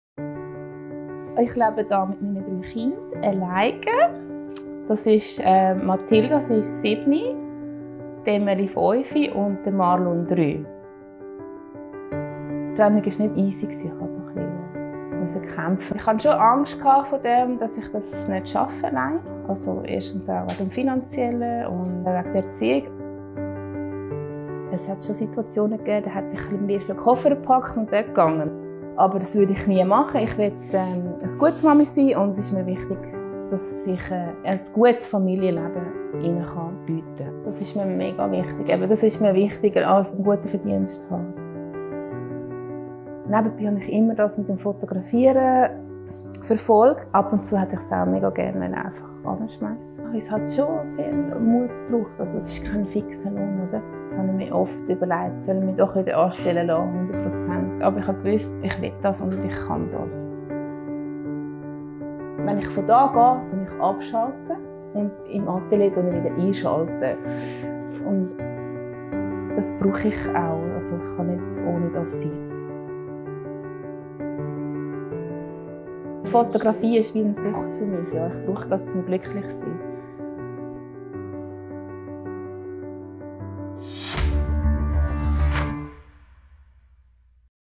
Ritratto di famiglia AXA: mompreneur Vanessa Bachmann
La fotografa Vanessa Bachmann cresce i suoi tre figli perlopiù da sola. Nel ritratto di famiglia AXA vi mostriamo come l’attività lavorativa indipendente l’aiuta a far fronte a questo compito di grande responsabilità.